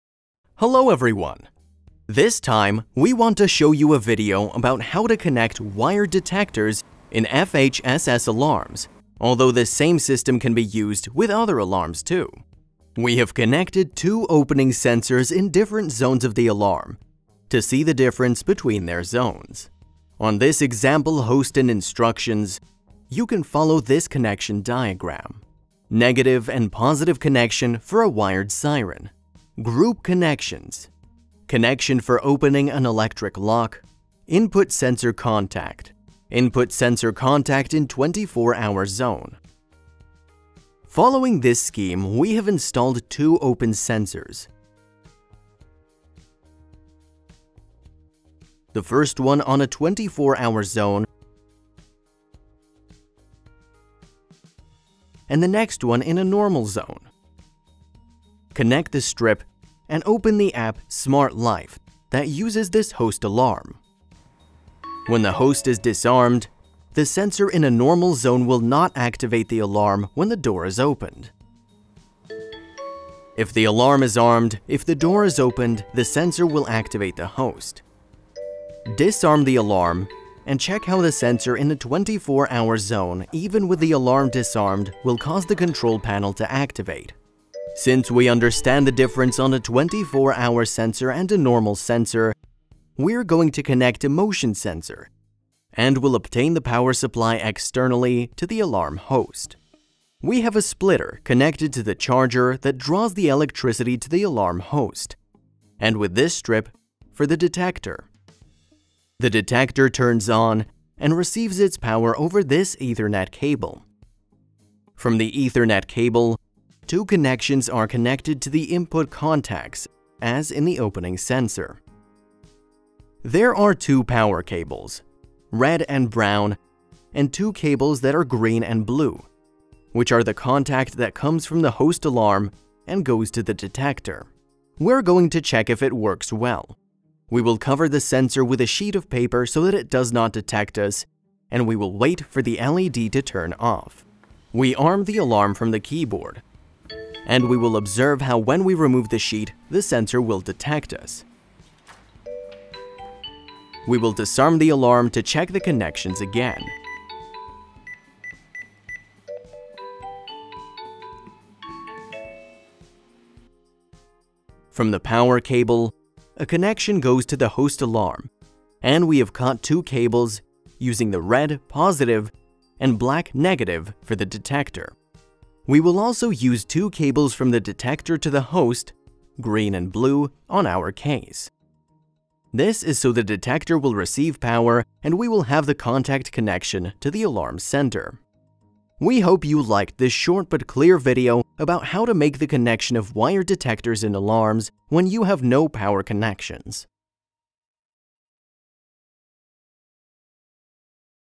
How to connect wired detectors in alarms with no power outlet. Door detectors, PIR sensor to host
In this video we want to show you how to install detectors in an alarm with no power outlet

We start by connecting 2 door-window opening detectors, and seeing the difference between a normal zone and a 24-hour zone

In the final part of the video we will connect a PIR motion detector wired to the alarm center and powered through the same cable from the power charger